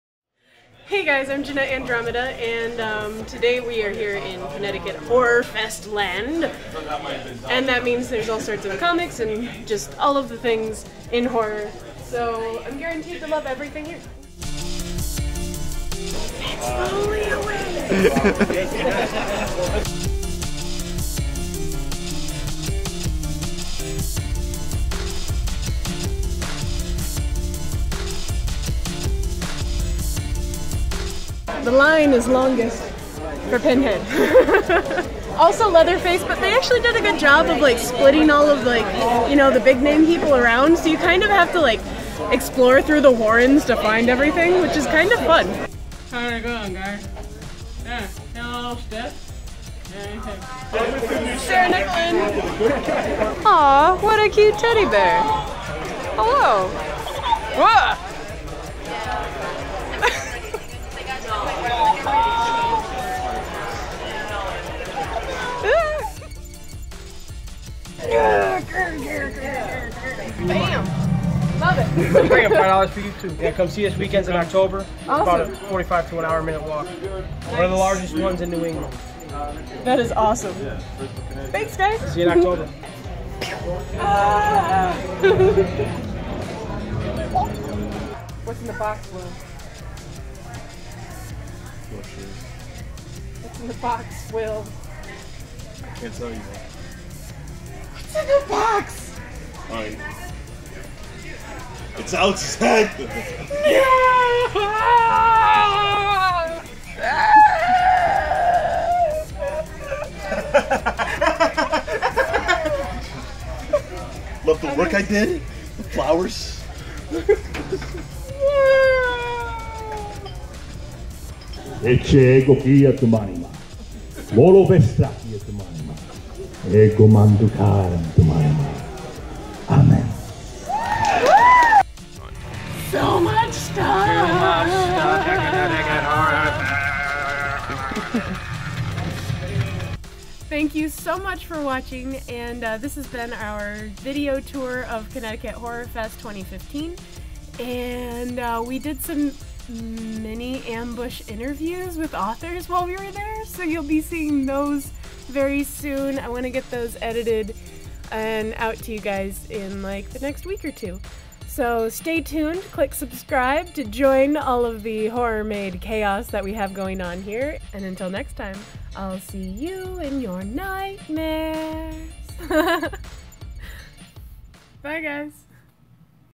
CT HorrorFest Convention Tour 2015
What's your favorite convention story?

Mine is from Haunting TV's first ever convention - Rock and Shock - when Geonna, Immortal Alexander and I were coming home from working in the dealer's den and we get cornered in the elevator at the motel by this Indian guy. He asks us all sorts of awkward questions with complete disregard for personal space.

First he points at Immortal Alexander and asks me, "Yours?"

I respond with, "Uh, yep, he's mine."

Elevator-man, "Very handsome, very handsome."

Me, "Yuuuup"

Elevator-man points at Geonna accusingly, "Are you twelve? Are you Twelve?!"

G (Who is no where near the age of 12 btw), "Uhhh. no."

Elevator-man, "Good."

*Ding* - we book it out of there, walking backwards towards our room.

Most awkward elevator ride ever.

One of my favorite actresses was there, Sarah Nicklin (from Nun of That, Disco Excorsist, and many other awesome B-horror movies.) You should totally check her stuff out

The music used in this video was By Cast Into Ruin. You can listen to even more songs over here:

We had a total blast and I, for one, can't wait to go back next year!

I wrote about more of the people we met on Horror Made

XOXO ~Jeanette